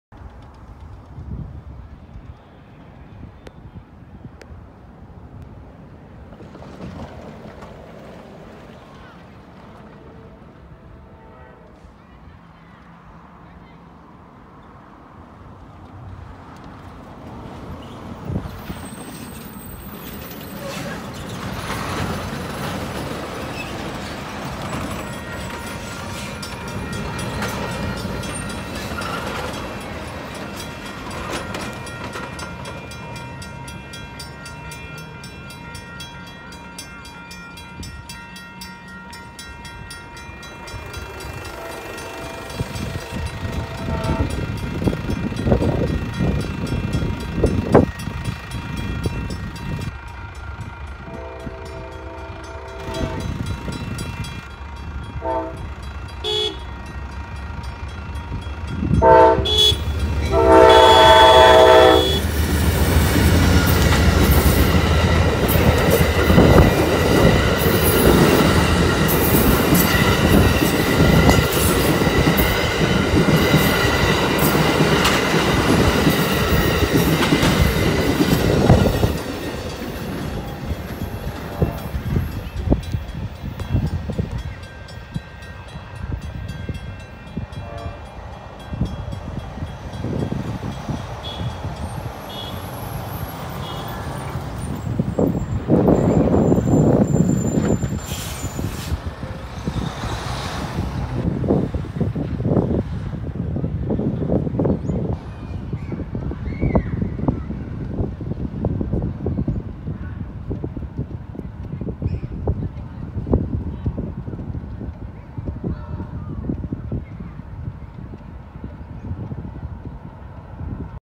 Amtrak Coast Starlight passes through Aromas w/ a friendly crew
While I was heading down to Watsonville, I spotted #11 running 1 hour and 45 minutes late passing the Highway 25 crossing at Miller and I took the opportunity to catch it at the Rogge Lane crossing in Aromas. You’ll notice as it rounds the curve and activates the crossing, one of the gates has a near miss with a quarry truck trying to beat it but after that I got a really cool shot of #11 passing through with a friendly crew and was also saluting a truck driver behind me.